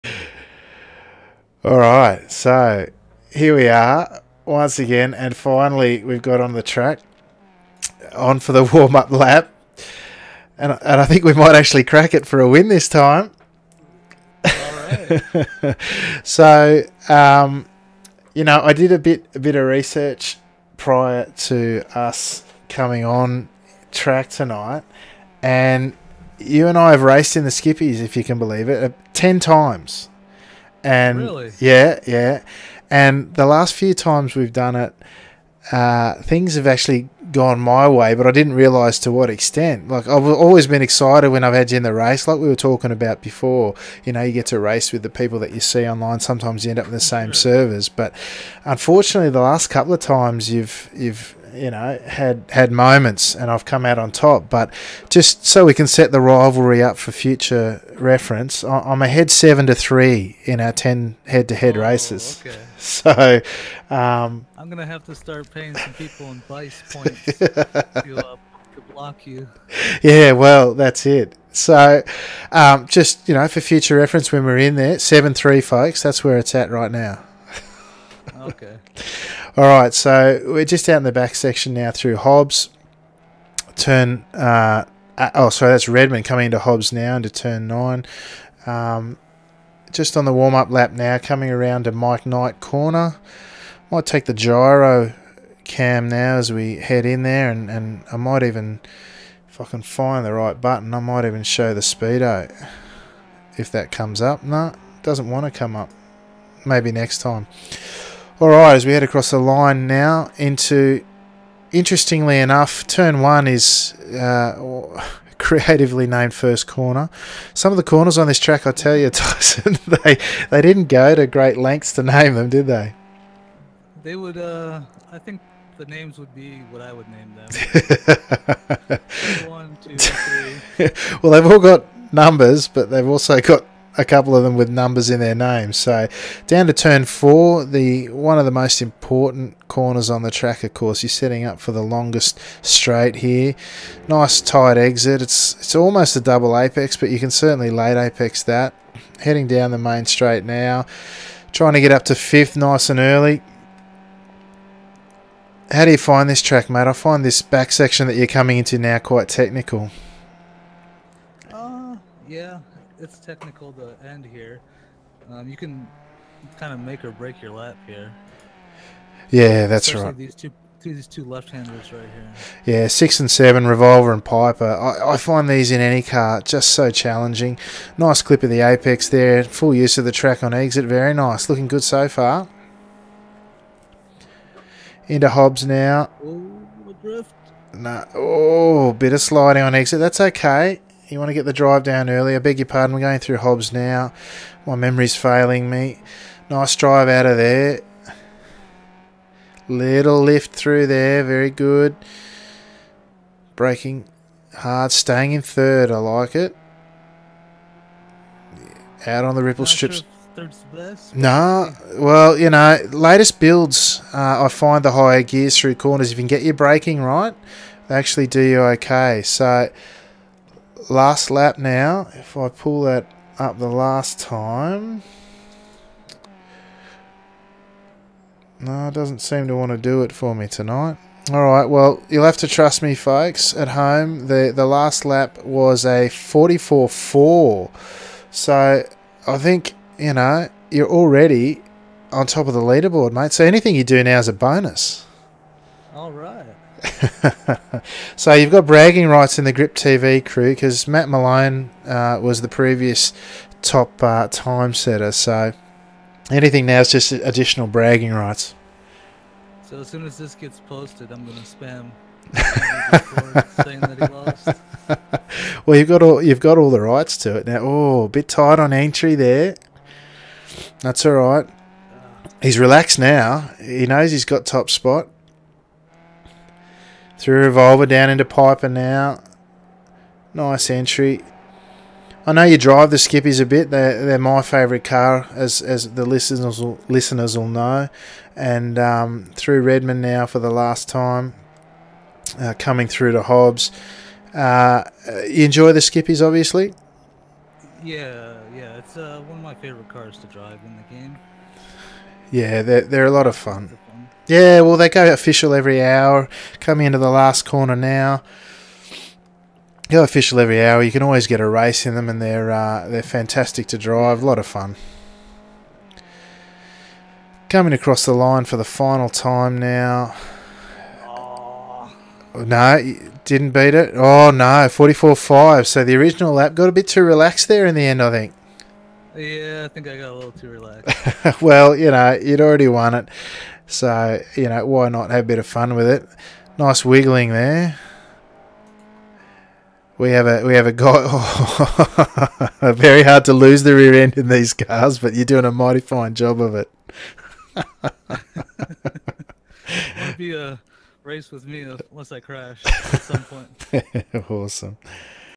Episode #006 - Tyson Meier
Tyson Meier tackles Okayama in the Skip Barber car for The Top Split Hotlaps Leader board.